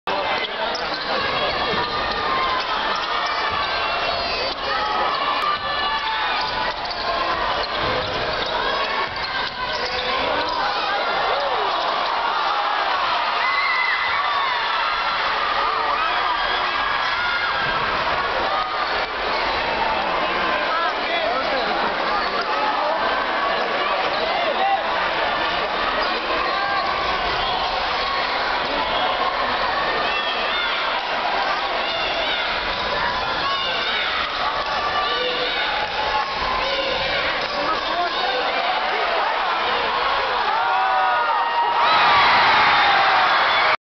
Another three pts
Battle of the Birds 2011 Wash vs Lowell Boys Varsity Basketball